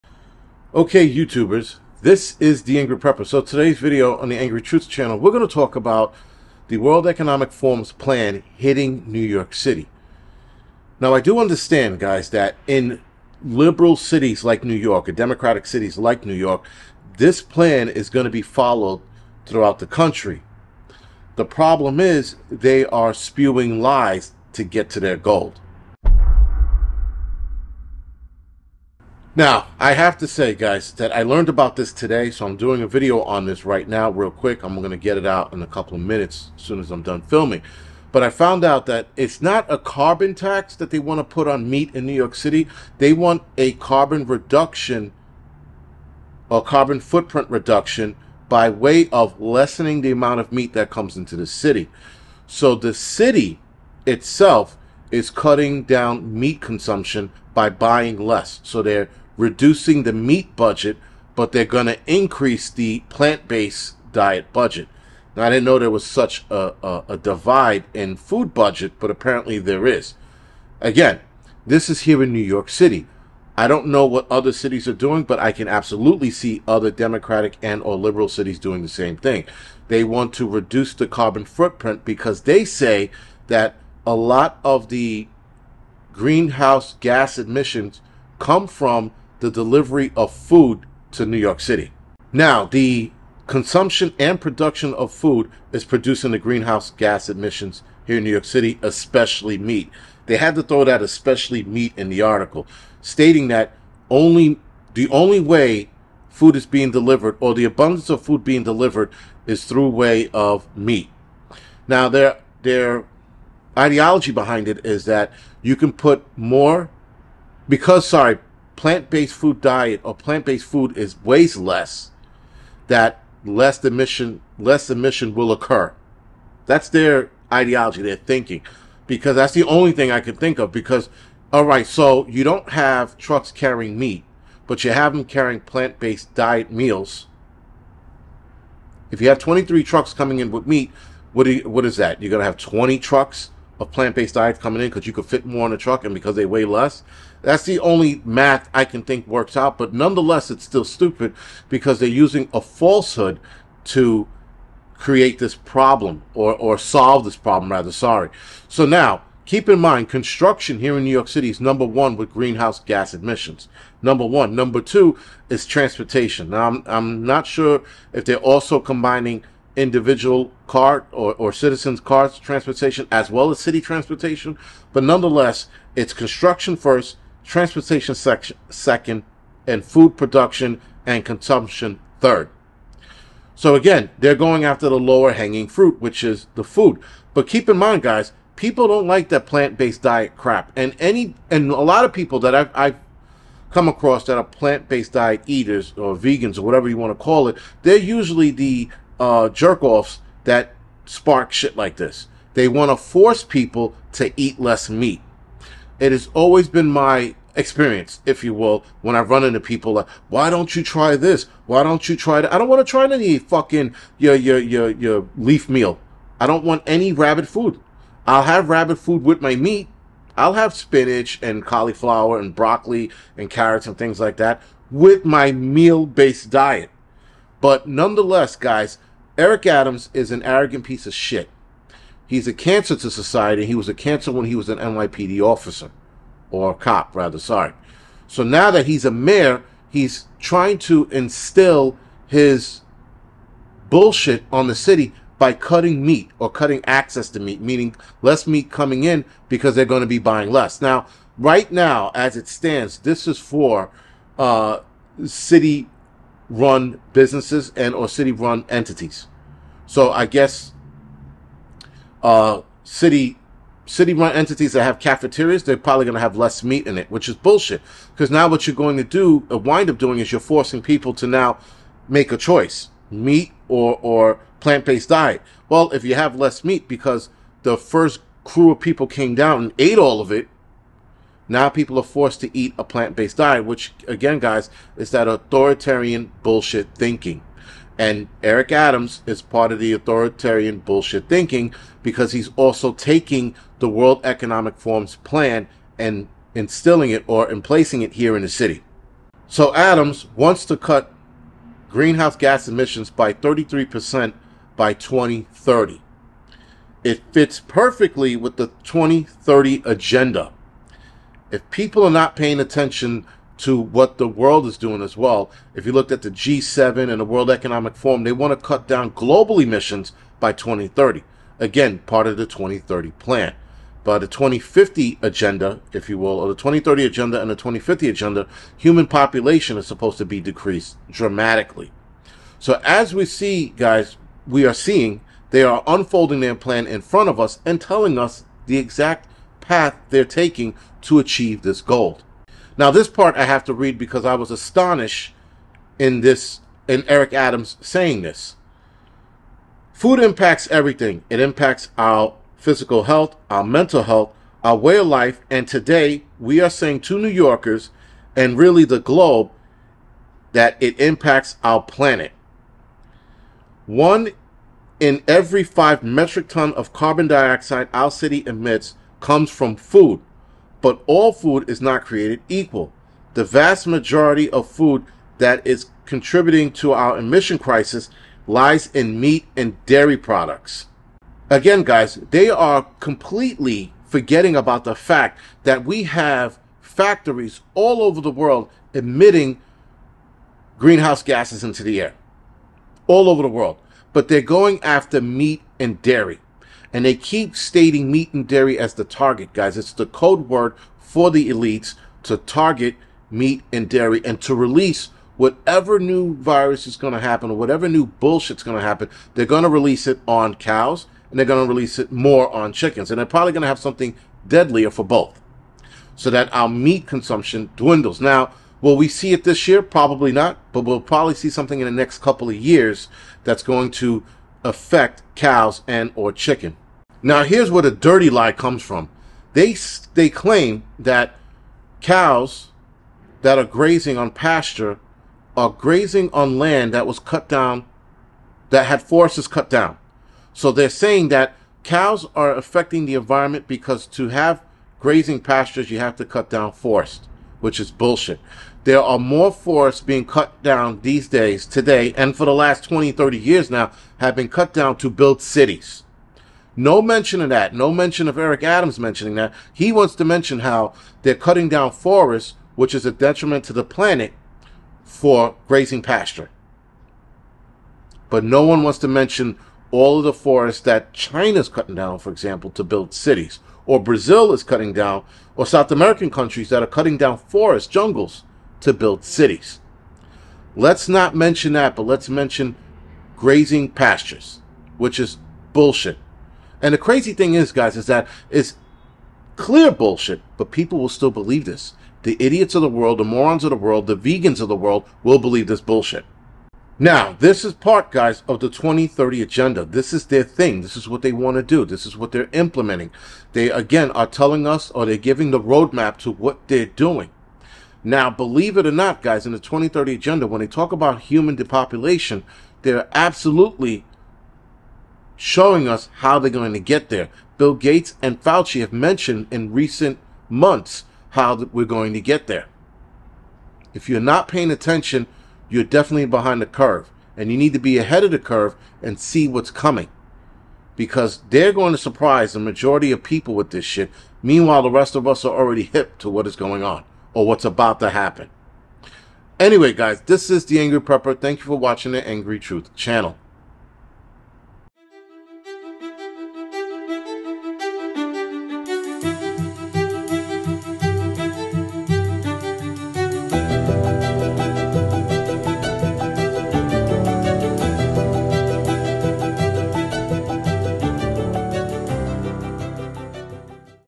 The WEF Plan Has Come To New York City?
On The Angry Truth Channel, we are going to talk about the WEF Plan Has Hit New York City?  This is right out of their play book. A lot of cities will start to implement this.  They are trying to block our meat consumption slowly.